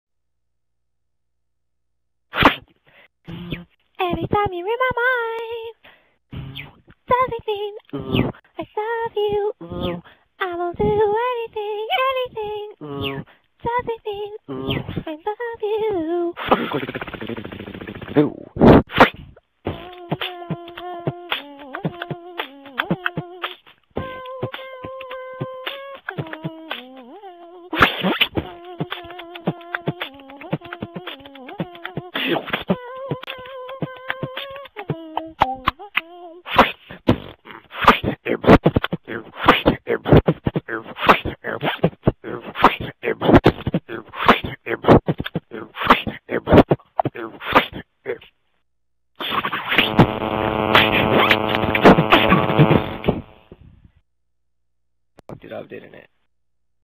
BEATBOXINGDANNY Episode 2
Here comes another one and even better. BAD ending when friends mic messes up but good for the rest